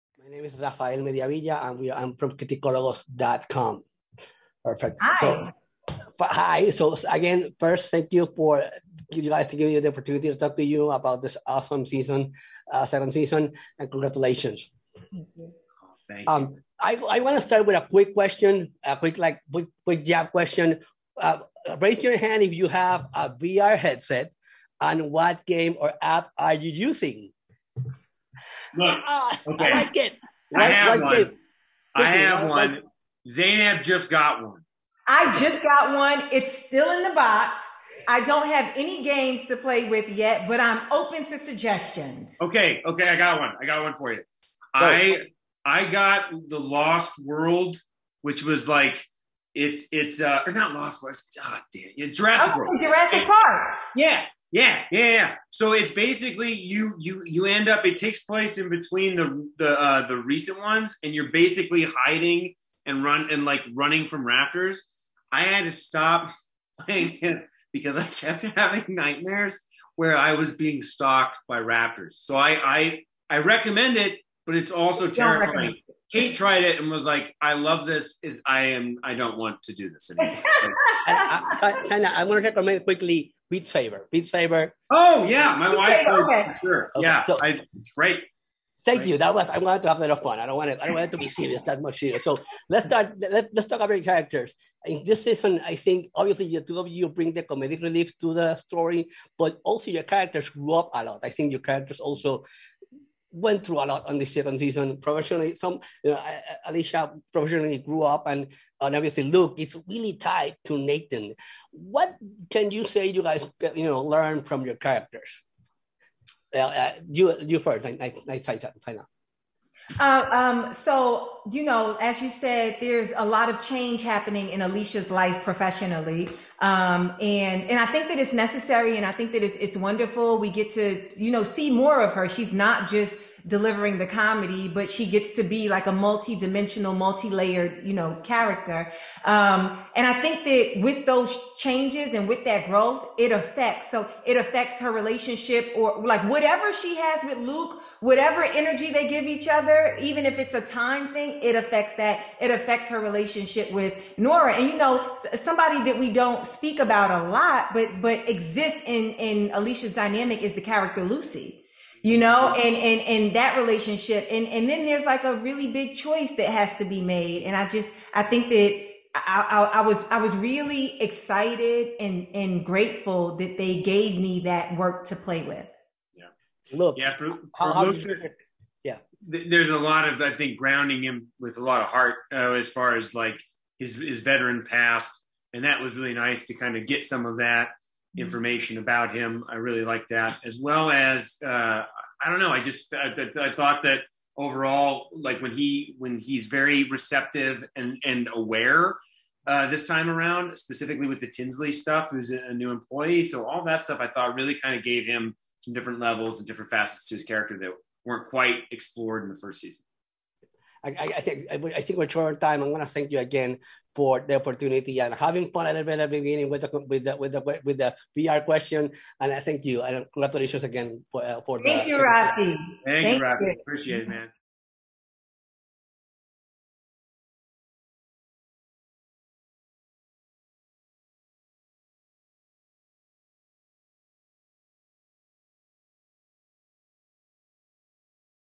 Interview w/ Kevin Bigley (Luke), & Zainab Johnson (Aleesha) From Prime Video "Upload" S2.
Interview w/ Kevin Bigley (Luke),  & Zainab Johnson (Aleesha) From Prime Video "Upload" S2, where we talked VR Headsets, & Character Development.

Upload is a sci-fi comedy series from Emmy Award-winning writer Greg Daniels (The Office, Parks and Recreation), set in a technologically advanced future where hologram phones, 3D food printers and automated grocery stores are the norm. Most uniquely, humans can choose to be “uploaded” into a virtual afterlife.

In Upload Season Two, Nathan is at a crossroads in his (after) life… his ex-girlfriend Ingrid has unexpectedly arrived to Lakeview hoping to strengthen their relationship, but his heart still secretly yearns for his customer service angel Nora. Meanwhile Nora is off the grid and involved with the anti-tech rebel group “The Ludds.” Season Two is packed with new near-future concepts, including Lakeview’s newest in-app digital baby program called, “prototykes,” and other satirical glimpses of the technological advances and headaches to come.

The series was created by Greg Daniels who also serves as executive producer along with Howard Klein. Series stars: Robbie Amell as “Nathan,” Andy Allo as “Nora,” Kevin Bigley as “Luke”, Allegra Edwards as “Ingrid,” Zainab Johnson as “Aleesha,” Owen Daniels as “A.I. Guy,” and Josh Banday as “Ivan,” and Andrea Rosen as “Lucy.”